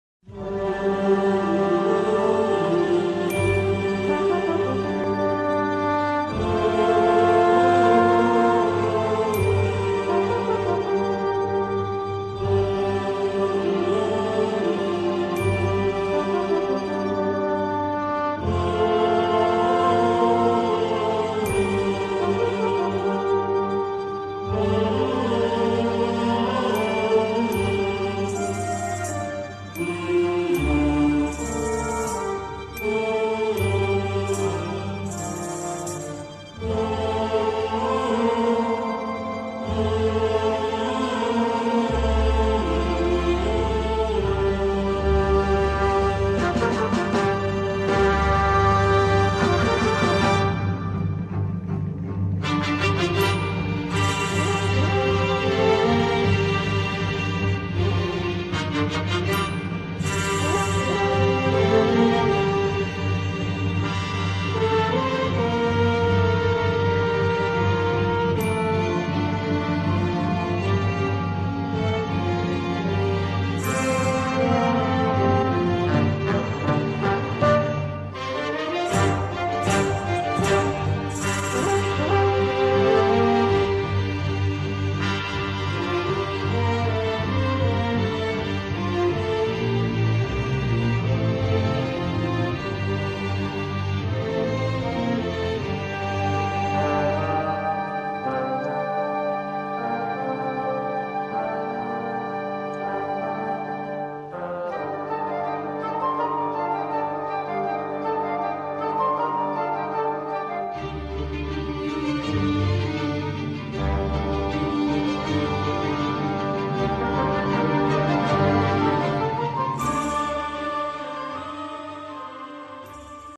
شعبون بی مخ کی بود؟
شعبون بی‌مخ یا شعبان جعفری را حتماً می‌شناسید. امروز سالروز مرگش است. زندگی‌اش را مرور کرده‌ایم.